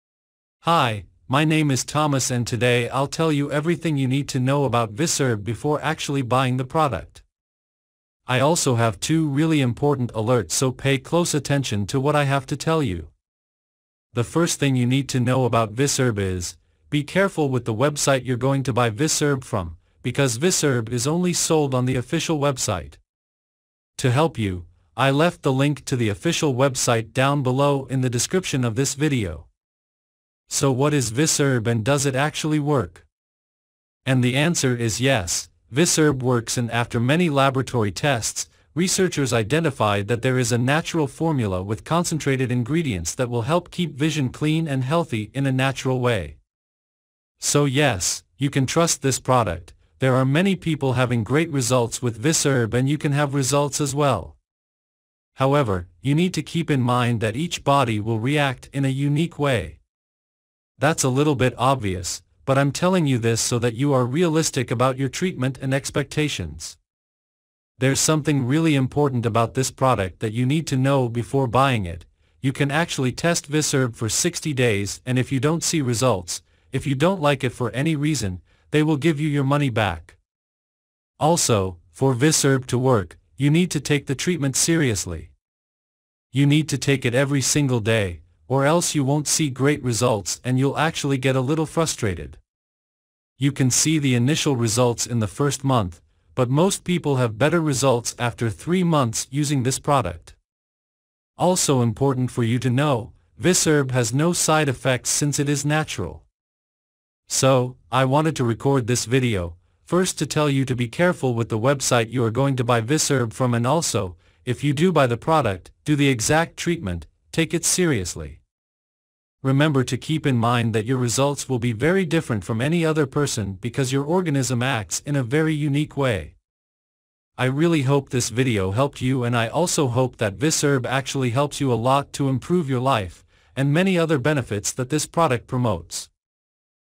Visherb - Visherb Review!!Visherb Supplement!!
I recorded this video because I saw that many people are doubtful if Visherb really works or if it is just another supplement that does not fulfill what promises, I hope to have helped with the video.

TAGS (IGNORE)